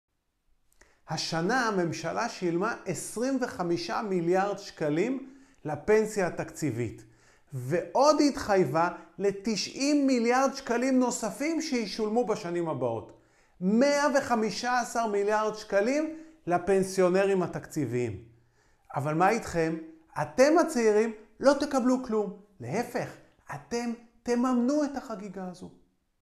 גם אתם רוצים פנסיה תקציבית.? לא תקבלו, אבל תשלמו
הממשלה שילמה 25 מיליארד שקלים לפנסיה תקציבית רק השנה ועוד התחייבה ל-90 מיליארד שקלים נוספים שישולמו בשנים הבאות. 115 מיליארד שקלים לפנסיונרים התקציביים. מה אתם תקבלו?